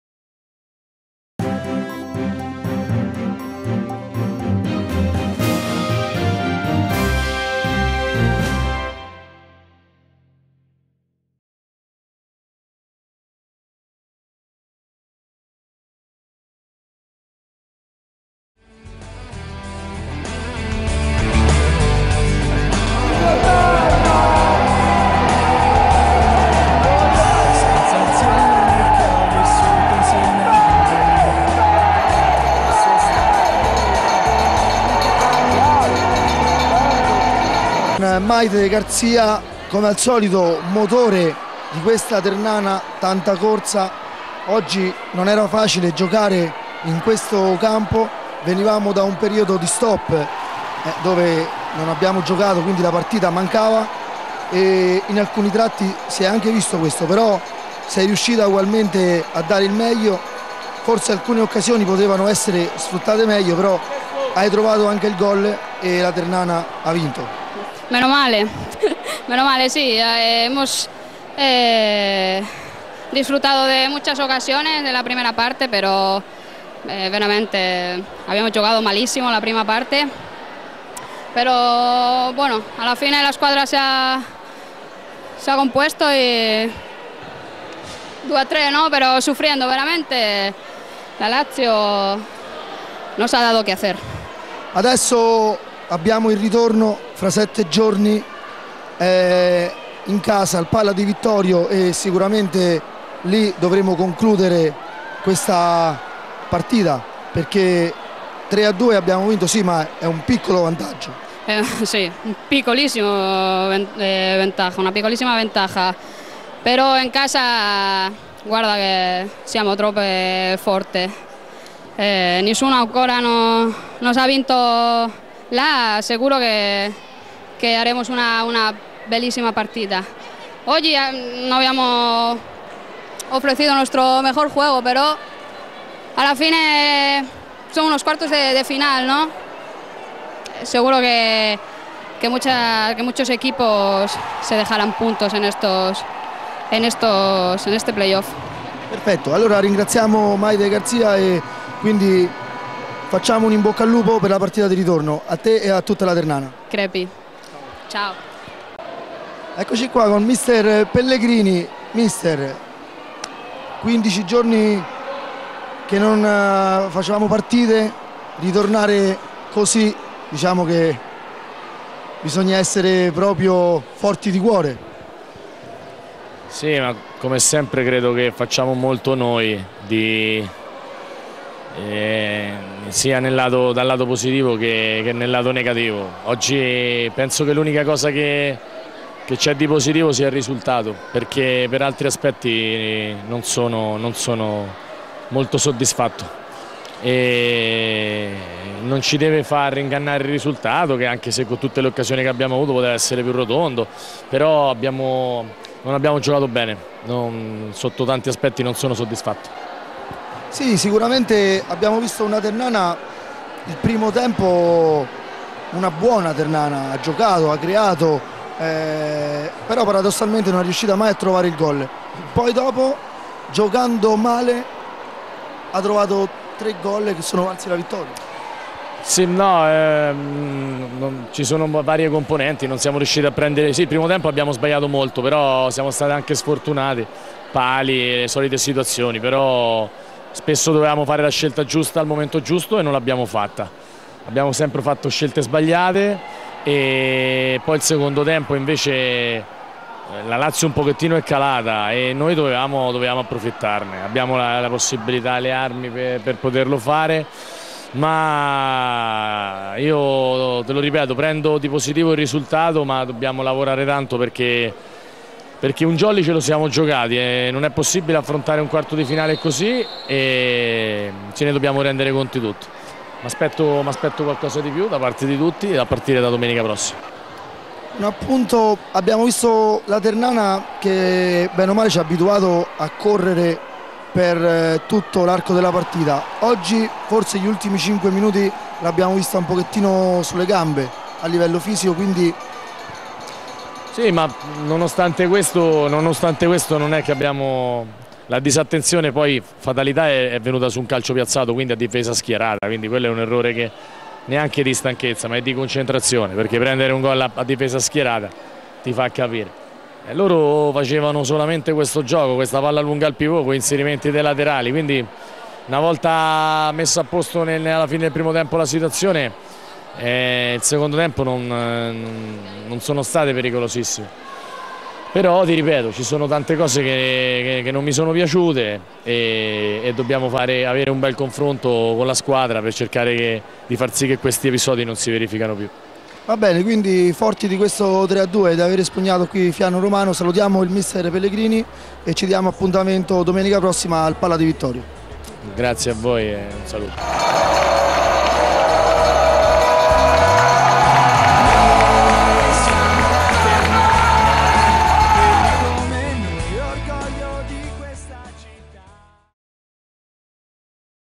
ONTV: Interviste post Lazio-Ternana Femminile (2-3)
Interviste ai protagonisti in rossoverde di Lazio-Ternana Femminile (2-3) partita d'andata dei quarti di finale playoff: Maite Garcia - Mister Pellegrini